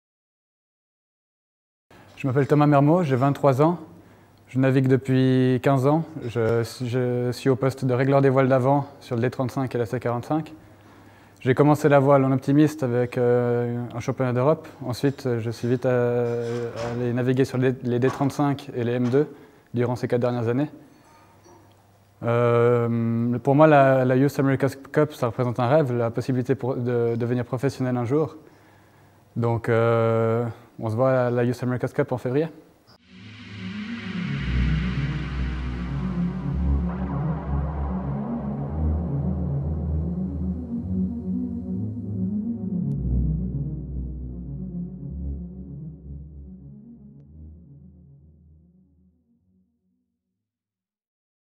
Thomas Mermod Trimmer - Team Tilt Sailing
Thomas Mermod talking about his sailing experience and explaining why he wants to participate in the Red Bull Youth America's Cup.